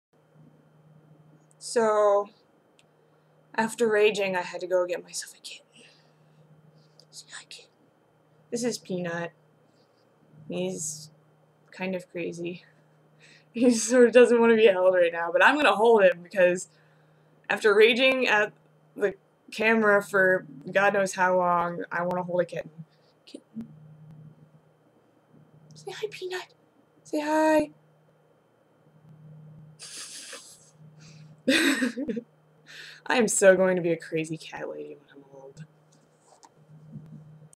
GRATUITOUS KITTEN
After I do Big Crits rage vids, I hold my kitten. He is adorable and defuses the rage.